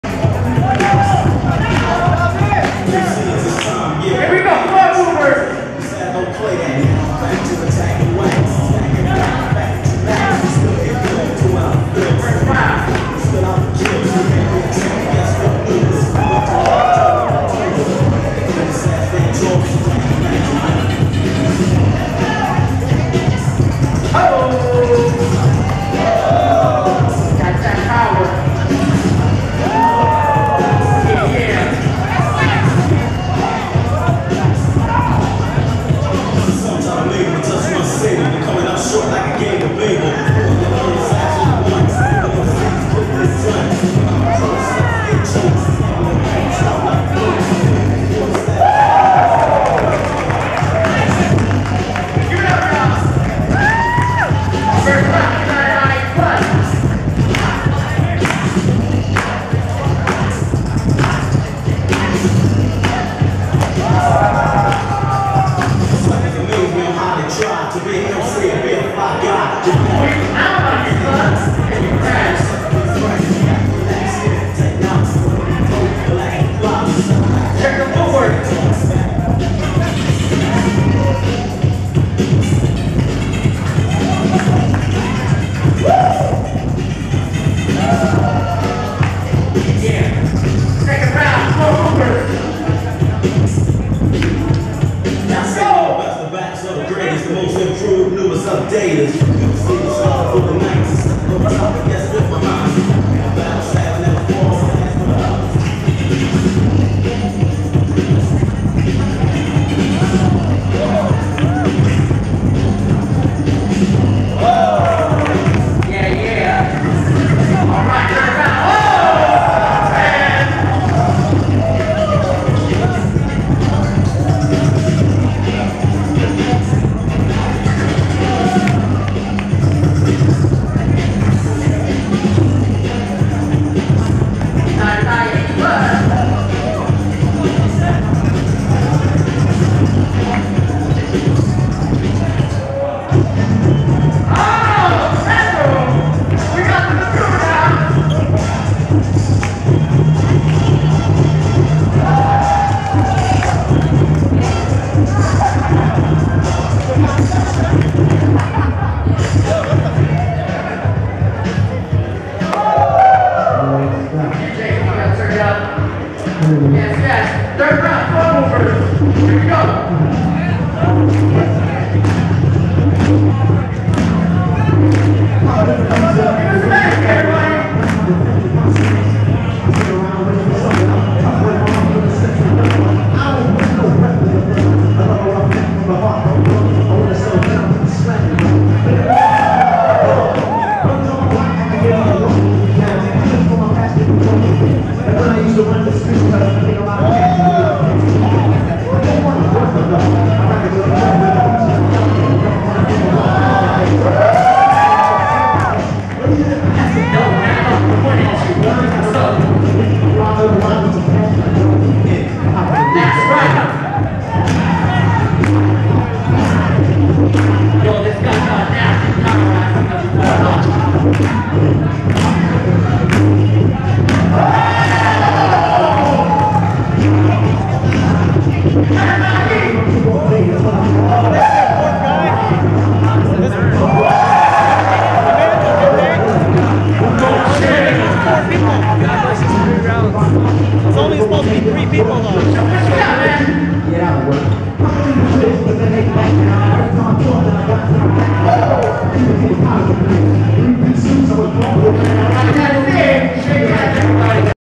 LAB 30th: Prelim Next Gen #1 U-Knighted IE vs. Floor Moverz
Prelim Next Generation B-Boy Crew Battles at the LA Breakers 30th Anniversary Jam at the Graffiti House in Los Angeles 2/23/2013.